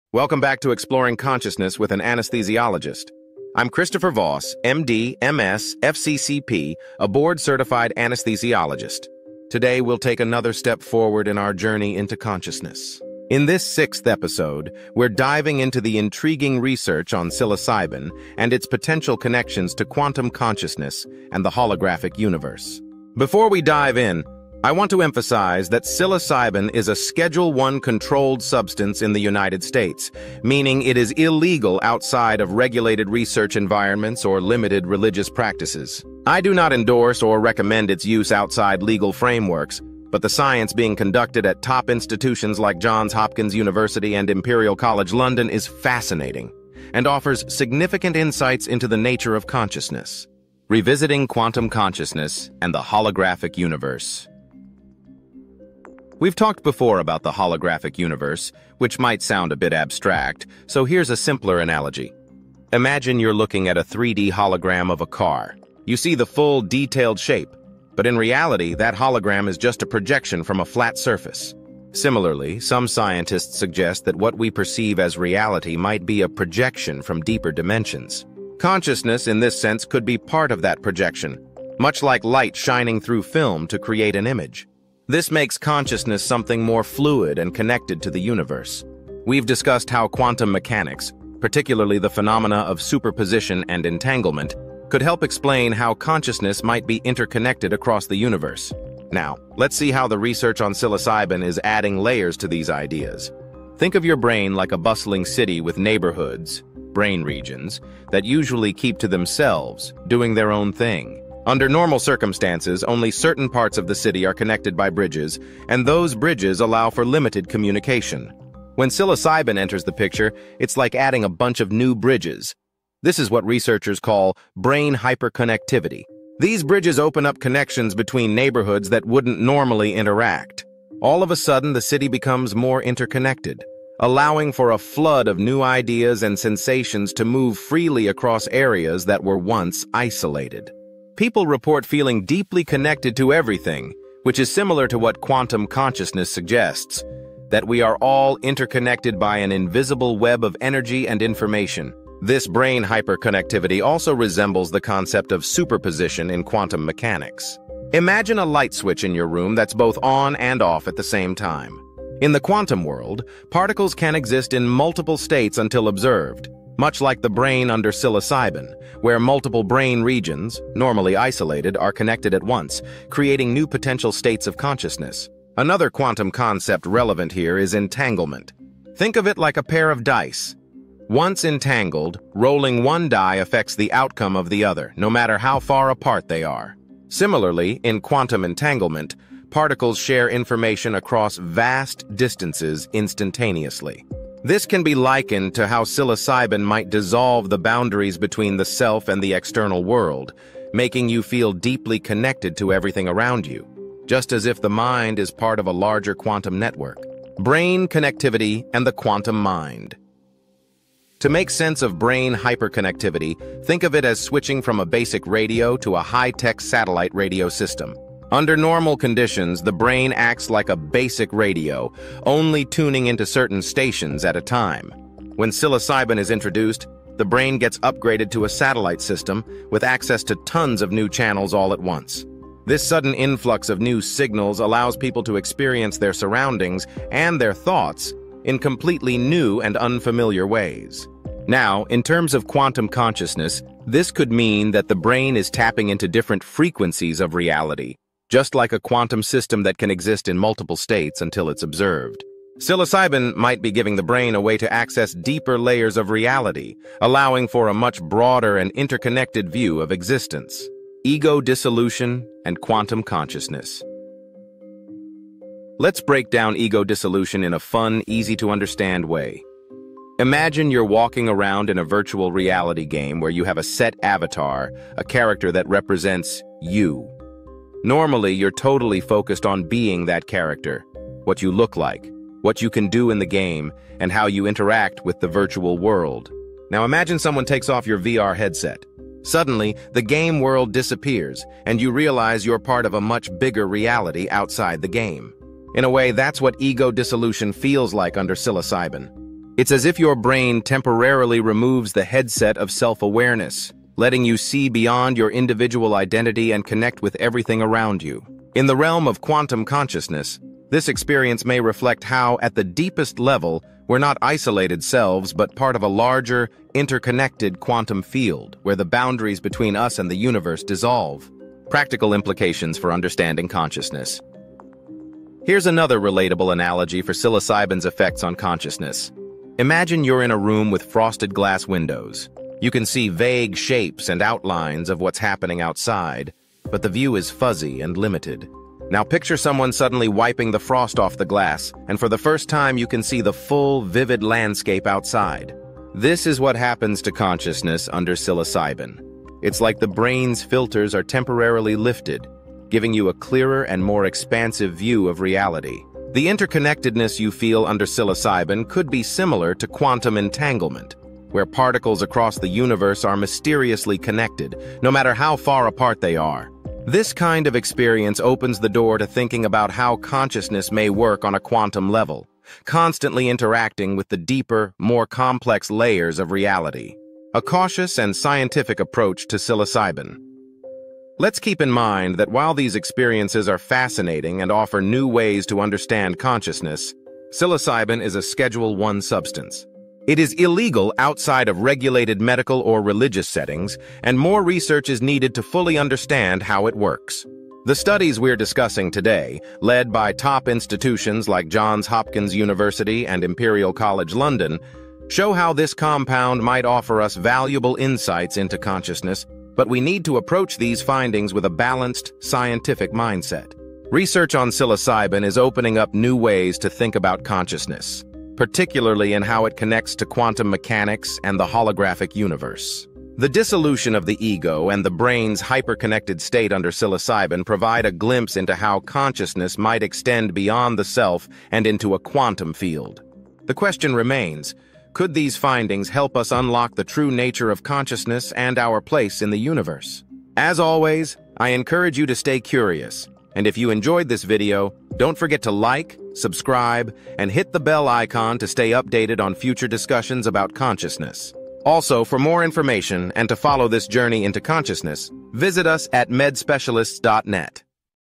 Unlocking the Quantum Mind: How Psilocybin is Changing Our Understanding of Consciousness
Exploring Consciousness with an Anesthesiologist - Episode 6: Psilocybin, Quantum Consciousness, and the Holographic Universe

Christopher Voss, MD, MS, FCCP
Board-certified in Anesthesiology, Critical Care Medicine, Neurocritical Care Medicine, Pain Medicine, and Addiction Medicine

References
1. Griffiths RR, Richards WA, Johnson MW, McCann UD, Jesse R. Psilocybin can occasion mystical-type experiences having substantial and sustained personal meaning and spiritual significance. Psychopharmacology (Berl). 2006 Jul;187(3):268-83.

2. Demertzi A, Barrett FS, Fort L, et al. Dynamic Functional Hyperconnectivity after Psilocybin Intake Is Primarily Associated with Oceanic Boundlessness. Biol Psychiatry Cogn Neurosci Neuroimaging. 2024.

3. Carhart-Harris RL, Friston KJ. The default-mode, ego-functions and free-energy: a neurobiological account of Freudian ideas. Brain. 2010 Apr;133(4):1265-83.